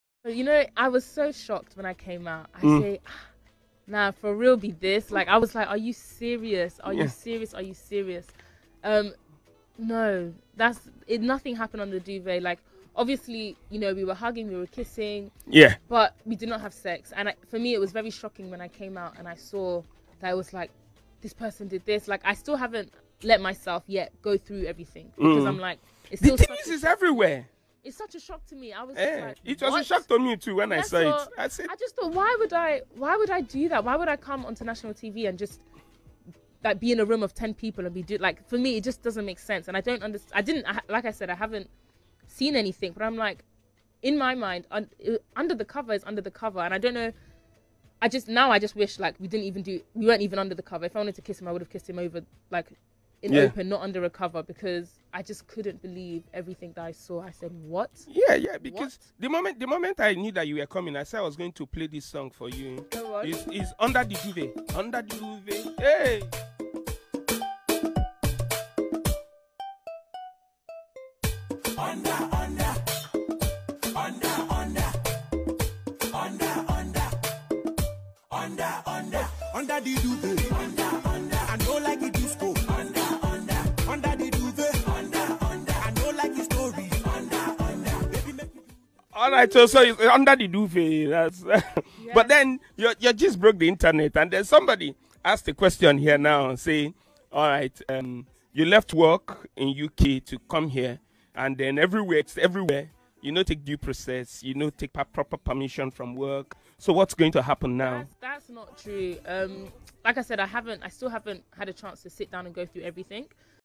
Yaw Interviews Khafi of Big Brother 9ja who claims she didnt have sex
Khafi of Big brother Naija denies sex in the house